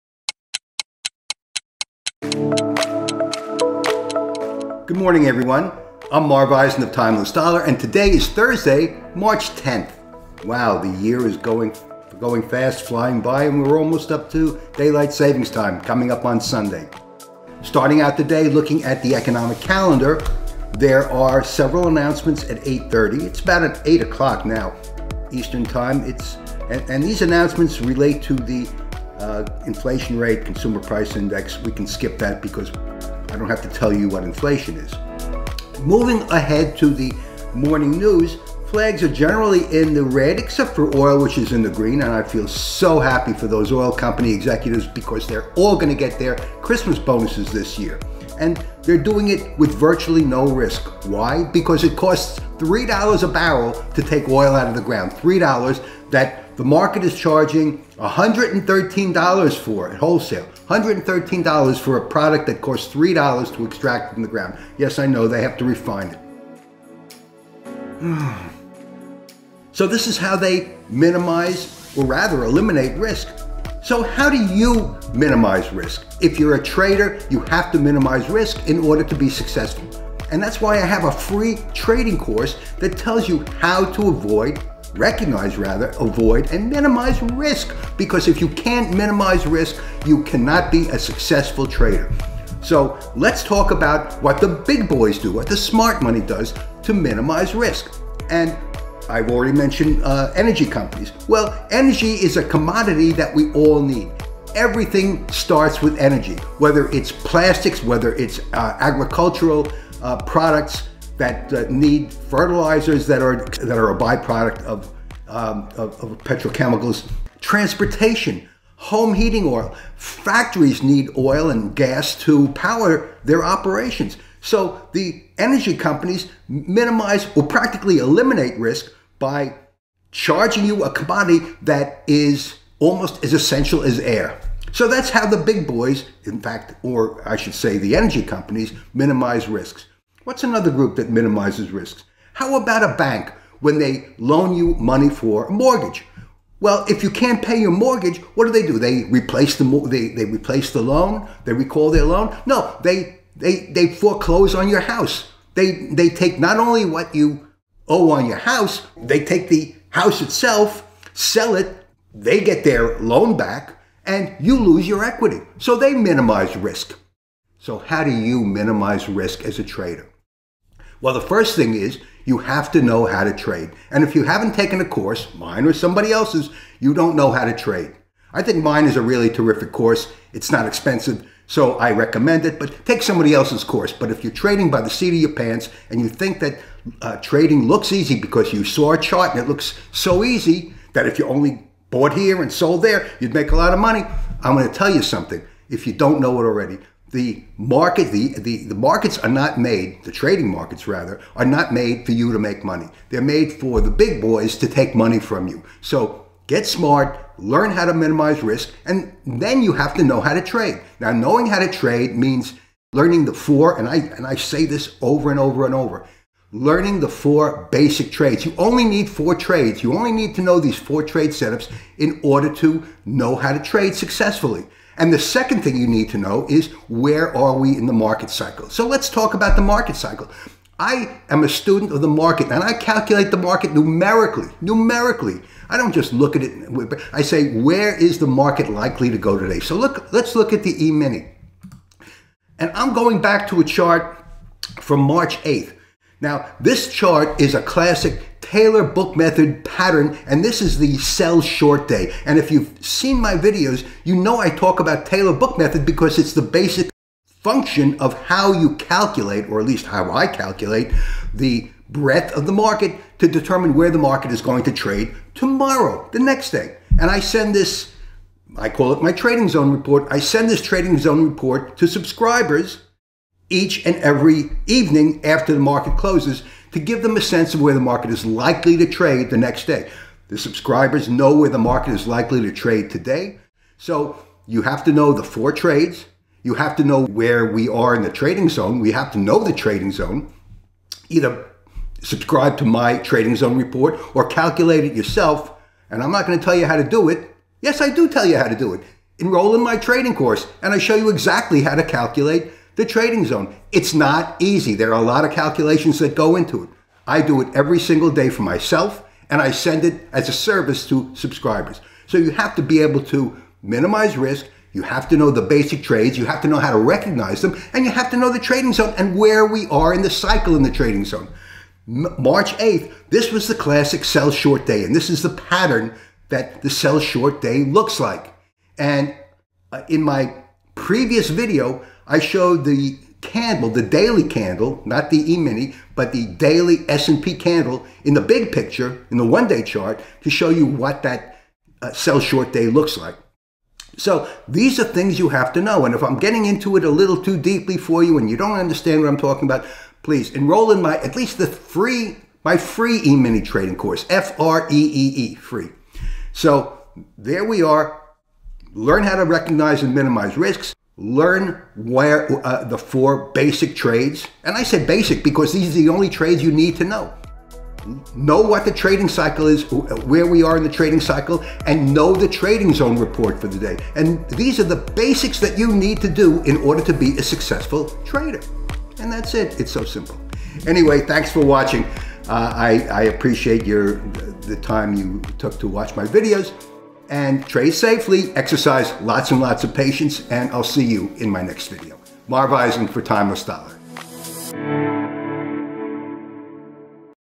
How Do YOU Minimize Trading Risk?
Knowing how to minimize risk is the very first thing every day trader needs to know. In fact, knowing how to minimize risk is more important than knowing how to trade because knowing how to trade without knowing how to minimize risk will not lead to long term trading success.
All big businesses that have become successful over the long term have risk minimization at the center of their business model. For example, when a bank makes a mortgage loan and the homeowner default on that loan, the bank minimizes risk by taking away the entire house, not just the portion that they loaned. For them, foreclosure laws protect them and minimize the risk. So how can traders minimize risk?
I consider risk minimization so important that I offer a free course that teaches about recognizing and minimizing risk. Of course, after learning how to minimize risk, it's important to learn how to trade properly. Part of learning how to trade properly is knowing the basic trades and also knowing the trading zone for the day.